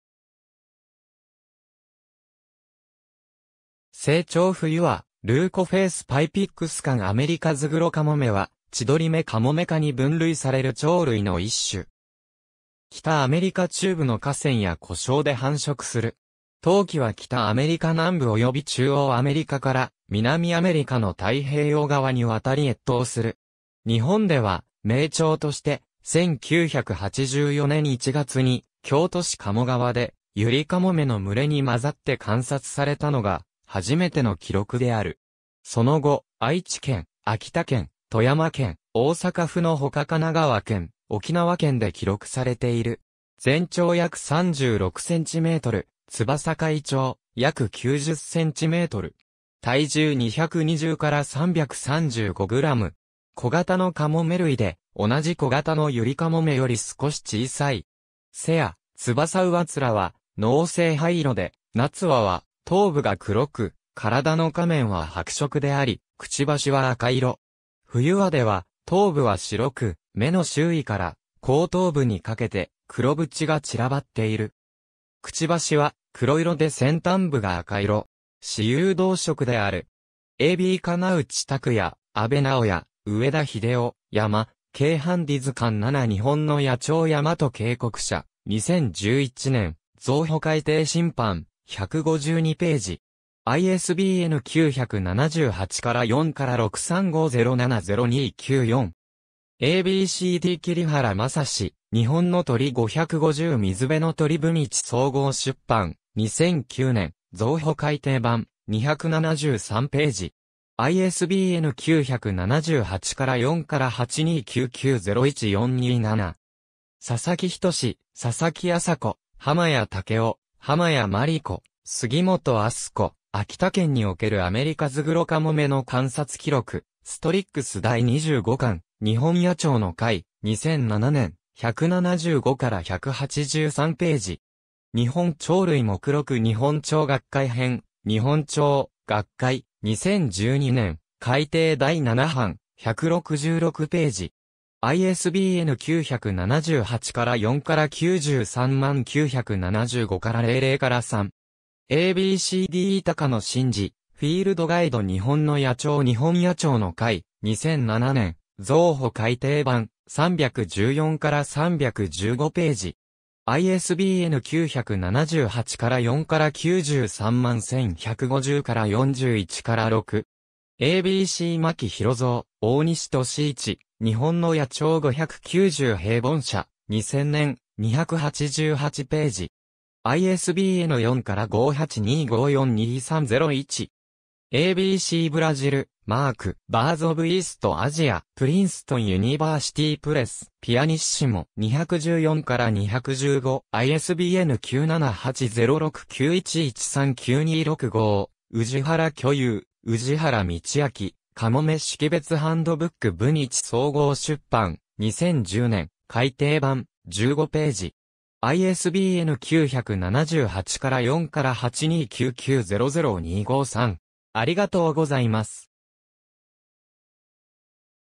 アメリカズグロカモメ
成鳥冬羽 Leucophaeus pipixcan アメリカズグロカモメ（アメリカ頭黒鷗、学名: Larus pipixcan）は、チドリ目カモメ科に分類される鳥類の一種。
北アメリカ中部（カナダ南部からアメリカ北部）の河川や湖沼で繁殖する。
冬期は北アメリカ南部および中央アメリカから南アメリカの太平洋側に渡り越冬する。
日本では迷鳥として、1984年1月に京都市賀茂川でユリカモメの群れに混ざって観察されたのが初めての記録である。
その後、愛知県（矢作川河口、1988年9月）、秋田県（八郎潟、1988年11月）、 富山県（黒部川河口1991年9月）、大阪府（大阪南港野鳥園、2000年11月）のほか神奈川県 （1998年6月）、沖縄県（与那国島、2006年7月）で記録されている。
全長約36cm(32-38cm)、翼開長約90cm(85-95cm)。
体重220-335g。
小形のカモメ類で、同じ小形のユリカモメより少し小さい。
背や翼上面は濃青灰色で、夏羽は頭部が黒く、体の下面は白色であり、嘴は赤色。
冬羽では頭部は白く、目の周囲から後頭部にかけて黒斑が散らばっている。
嘴は黒色で先端部が赤色。
雌雄同色である。
^ a b 叶内拓哉、安部直哉・上田秀雄『山溪ハンディ図鑑7 日本の野鳥』山と溪谷社、2011年、増補改訂新版、152頁。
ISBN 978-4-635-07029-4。
^ a b c d 桐原政志『日本の鳥550 水辺の鳥』文一総合出版、2009年、増補改訂版、273頁。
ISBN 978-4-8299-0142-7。
^ 佐々木均・佐々木あさ子・浜谷武雄・浜谷まり子・杉元明日子「秋田県におけるアメリカズグロカモメの観察記録」『Strix』第25巻、日本野鳥の会、2007年、 175-183頁。
^ 『日本鳥類目録』日本鳥学会（目録編集委員会）編、日本鳥学会、2012年、改訂第7版、166頁。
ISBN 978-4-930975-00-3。
^ a b c d e 高野伸二『フィールドガイド 日本の野鳥』日本野鳥の会、2007年、増補改訂版、314-315頁。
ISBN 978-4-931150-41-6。
^ a b c 真木広造、大西敏一『日本の野鳥590』平凡社、2000年、288頁。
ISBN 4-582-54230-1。
^ a b c Brazil, Mark (2009). Birds of East Asia. Princeton University Press. pp. 214-215. ISBN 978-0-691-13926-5 ^ 氏原巨雄・氏原道昭『カモメ識別ハンドブック』文一総合出版、2010年、改訂版、15頁。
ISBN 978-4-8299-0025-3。